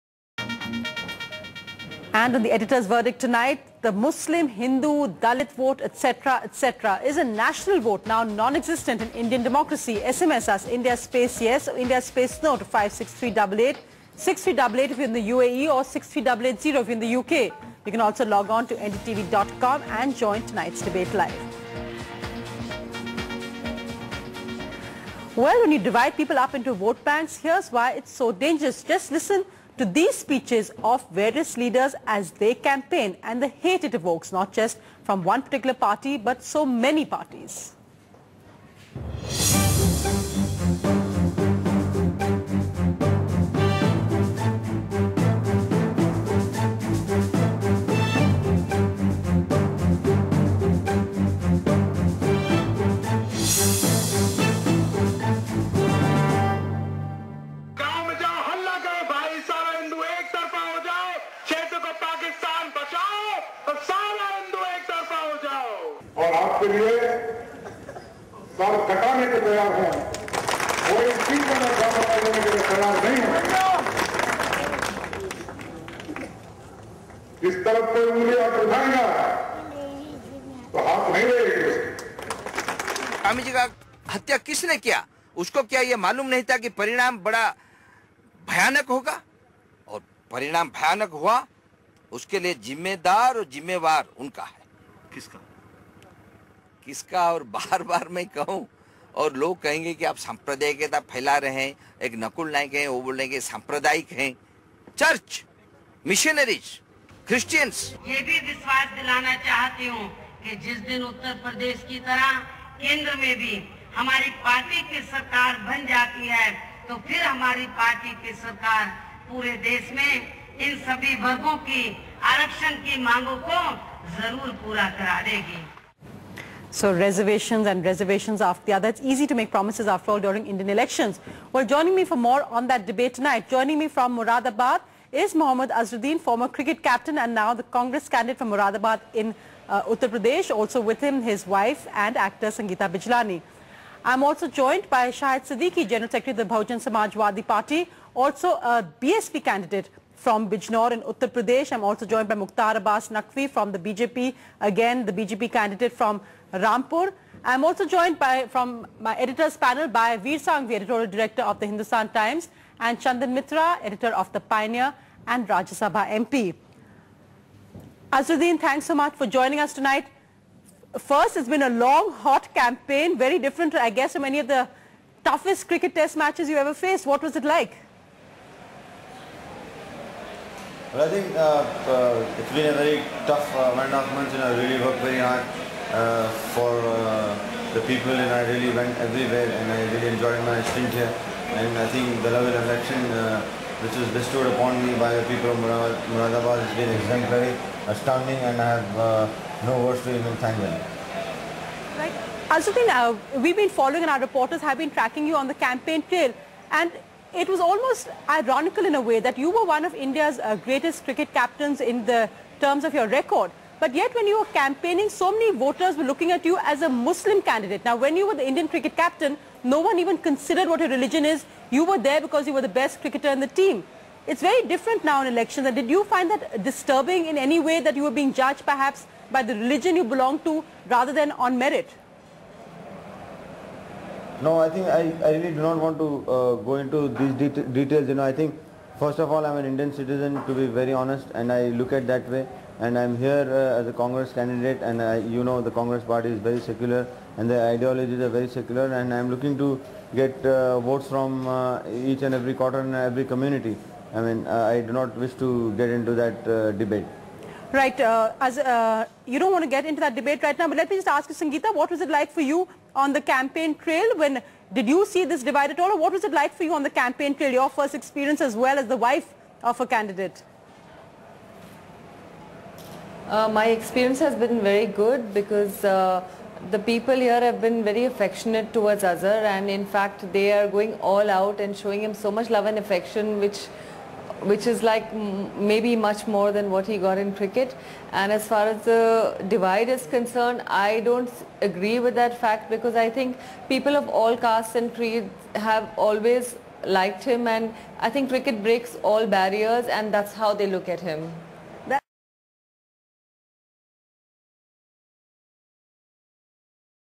Is 'national' vote now non-existent in Indian democracy?
Vir Sanghvi, Mukhtar Abbas Naqvi, Chandan Mitra and Shahid Siddiqui discuss the issue with Sonia Singh.